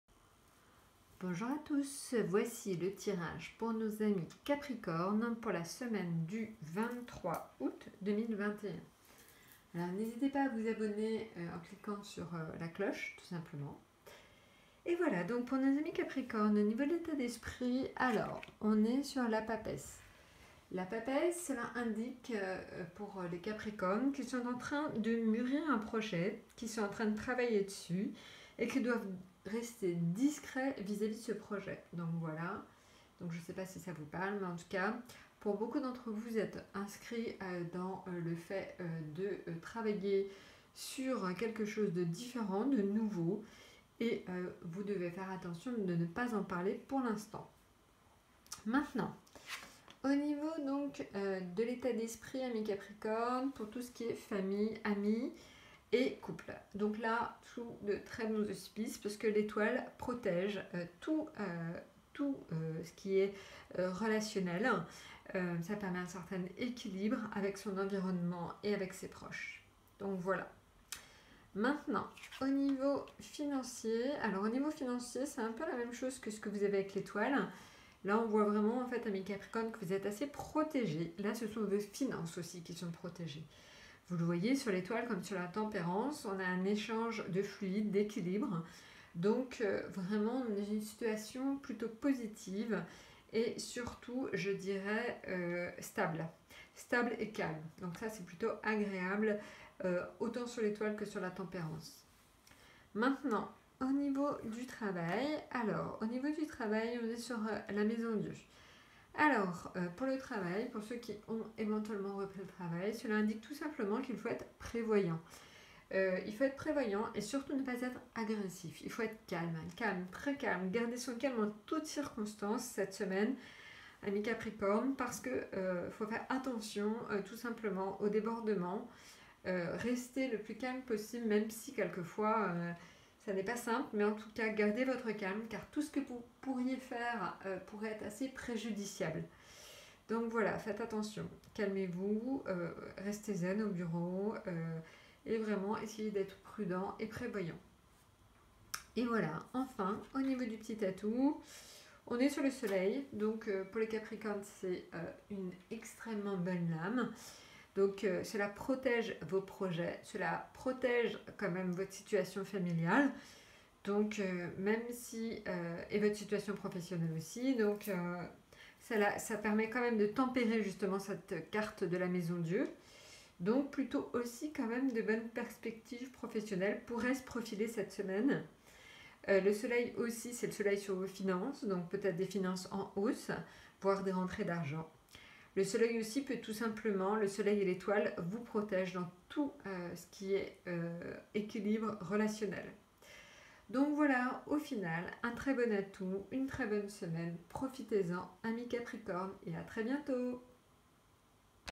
TIRAGE pour les CAPRICORNES pour la semaine du 23 août 2021 ⭐️🌟✨
Un tirage avec bcp de protection et normalement votre atout qui vous fera passer une excellente semaine